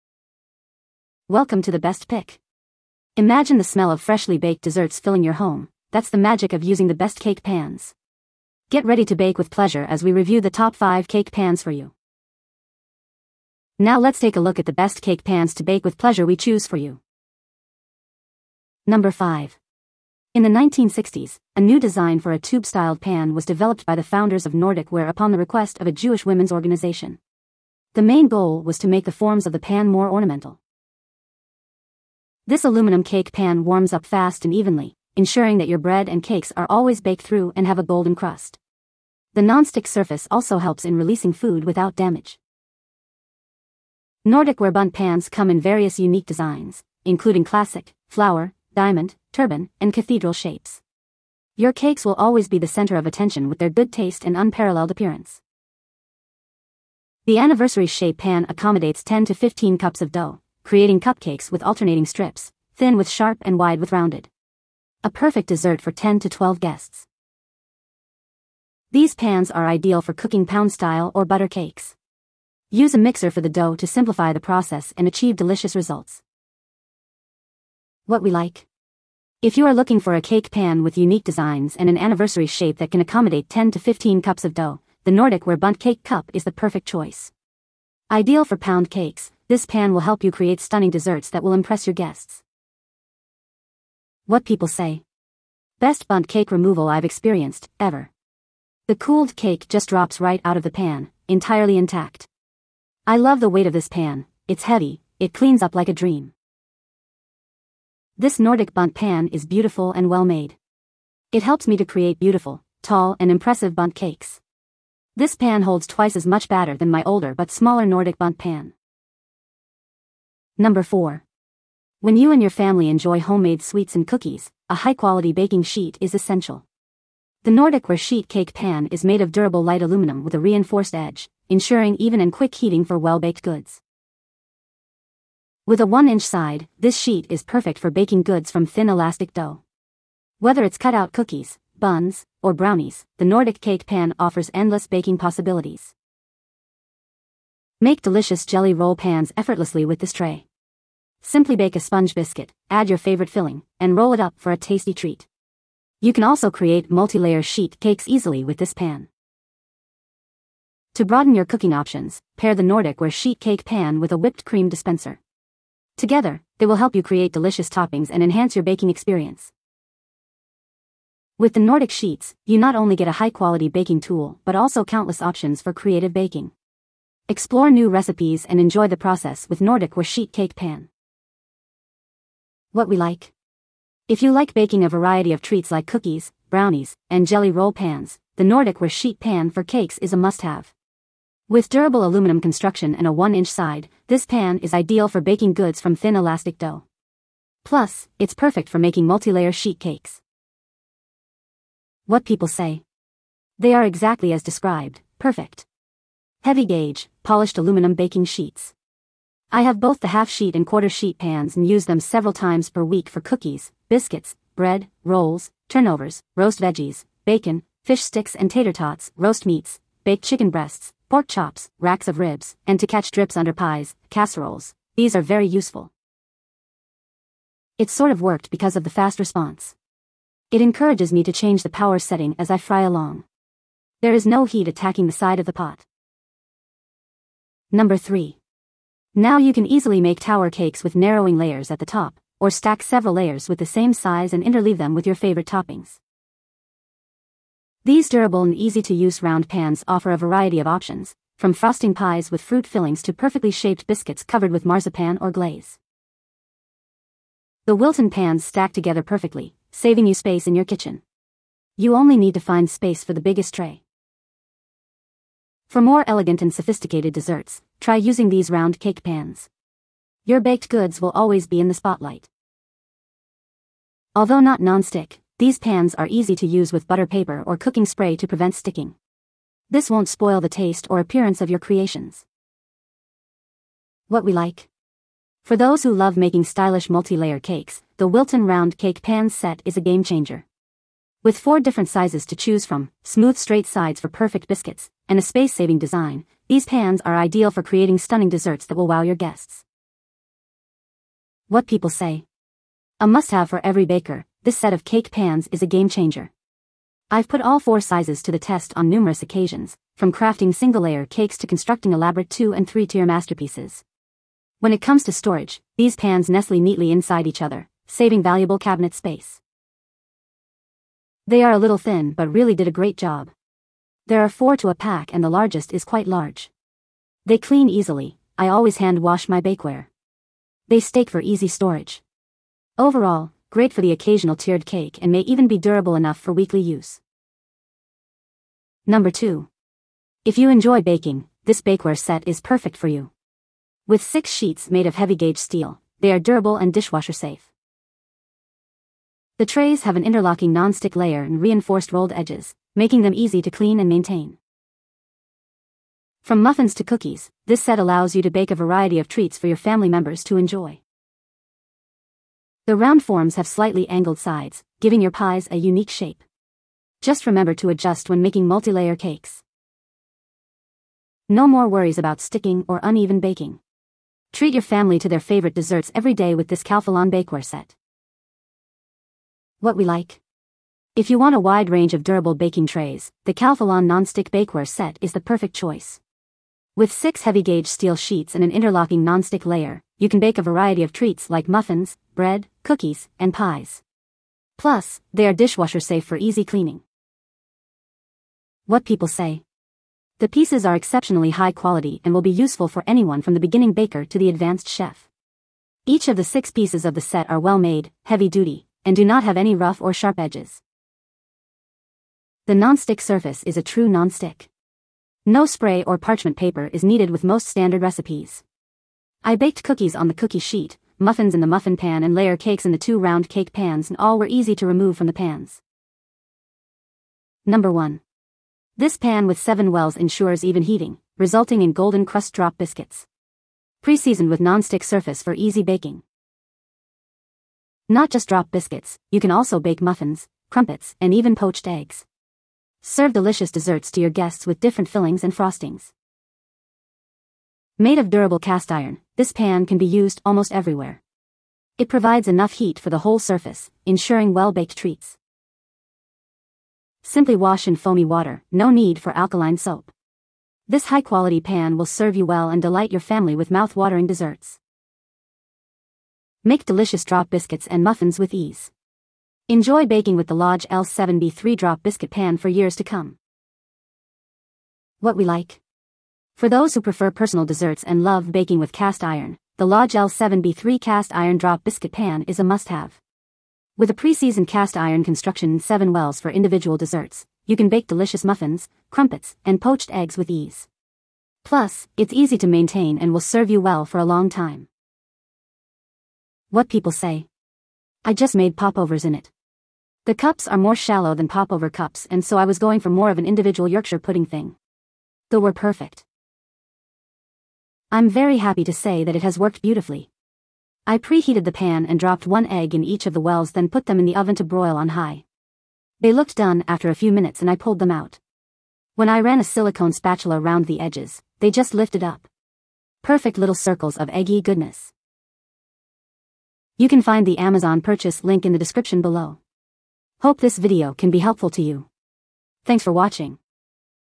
The Best Cake Pans to Bake With Pleasure in 2024 - Must Watch Before Buying!
The Best Cake Pans to Bake With Pleasure Shown in This Video:
5. ►Nordic Ware Bundt Pan
4. ►Nordic Ware Sheet Cake Pan
3. ►Wilton Round Cake Pans
2. ►Calphalon Nonstick Bakeware Set
1. ►Lodge L7B3 Drop Biscuit Pan

Table of Contents:
0:00​​​ - Introduction
00:17 - Nordic Ware Bundt Pan
01:59 - Nordic Ware Sheet Cake Pan
03:56 - Wilton Round Cake Pans
05:37 - Calphalon Nonstick Bakeware Set
07:06 - Lodge L7B3 Drop Biscuit Pan

Imagine the smell of freshly baked desserts filling your home - that's the magic of using the best cake pans! Get ready to bake with pleasure as we review the top five cake pans for you.

32d6